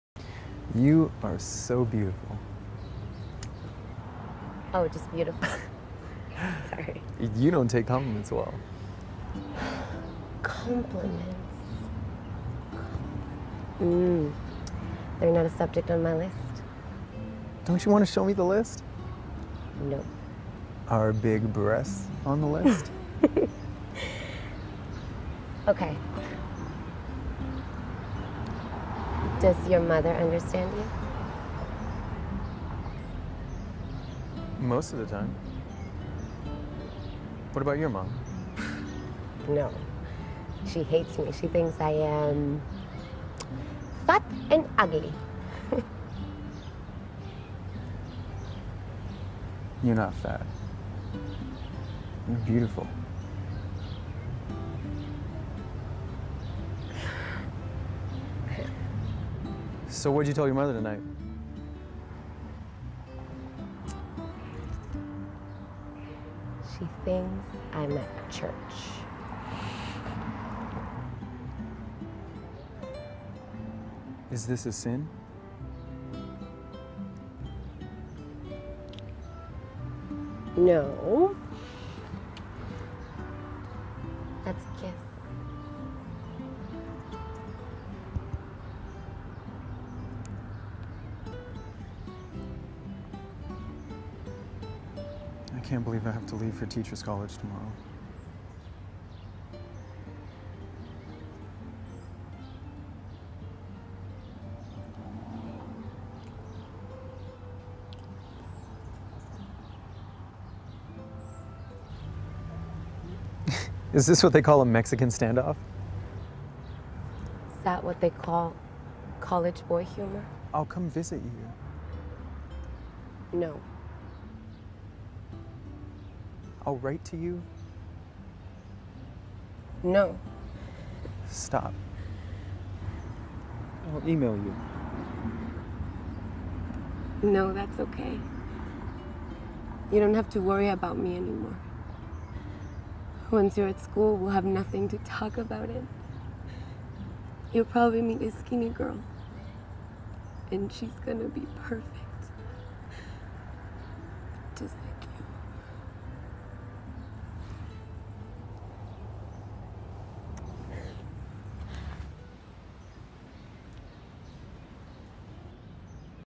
Scene from Real Women Have Curves
Shelly Ro and Anthony Gonzales perform Real Women Have Curves in the Industry Master Class at Baron Brown Studio.
The Baron Brown Studio specializes in the Meisner Technique. All levels of experience are considered for admission.